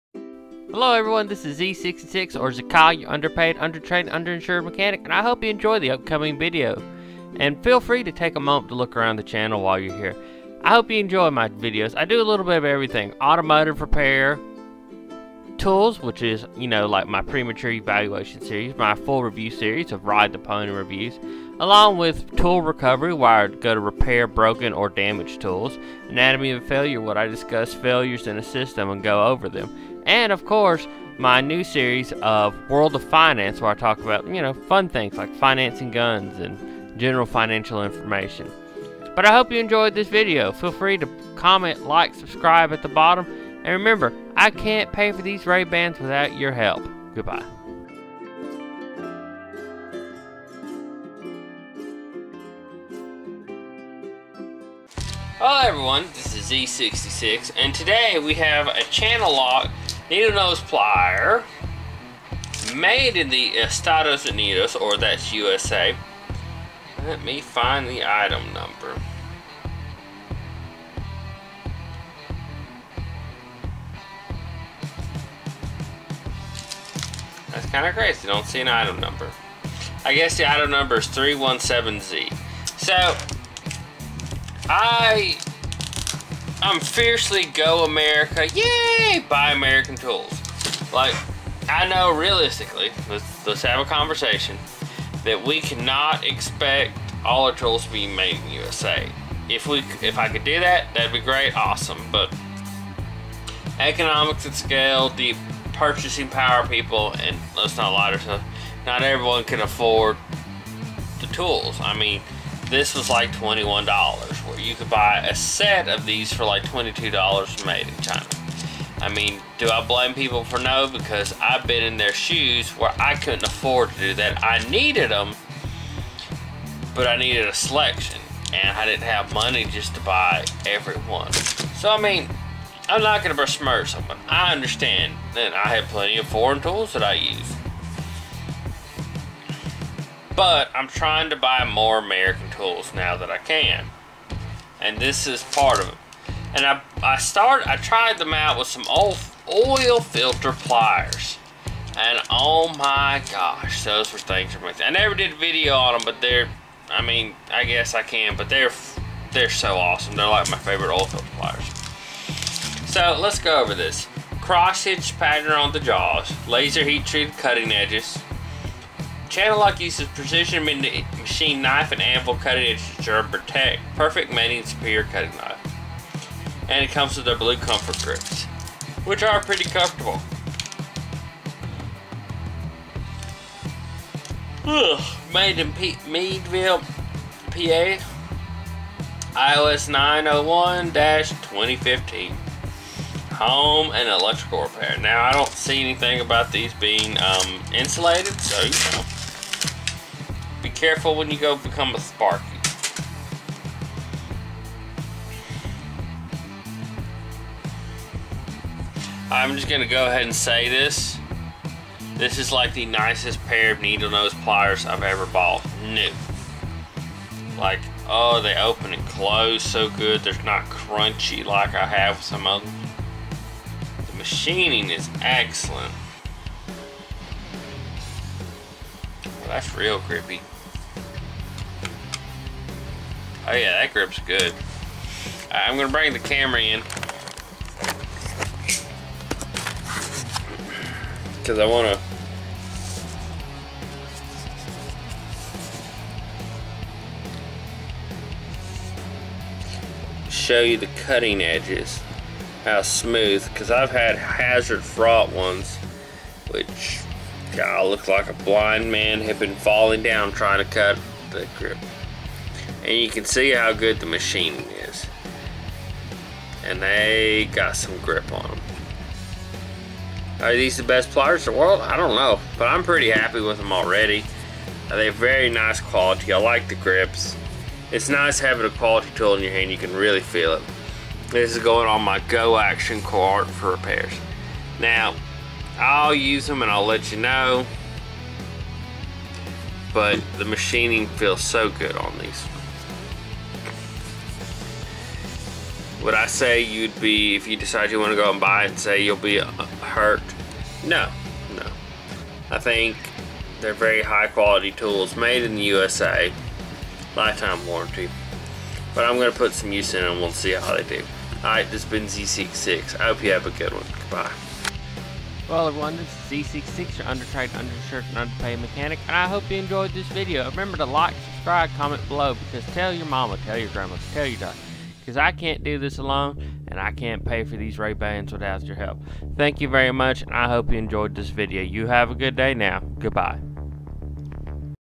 Premature Evaluation #104 Channellock 7- 1/2 Long Needle Nose Pliers
In this video, I give my opinions about these channel lock pliers.